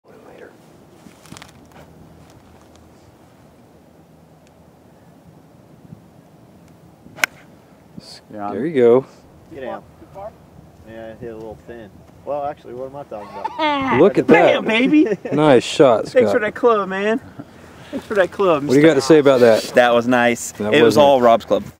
Nipper on the 17th Green at Sawgrass
Nipper hits the golf ball close to the pin on the 17th hole at TPC Sawgrass. Taken January 9th, 2010.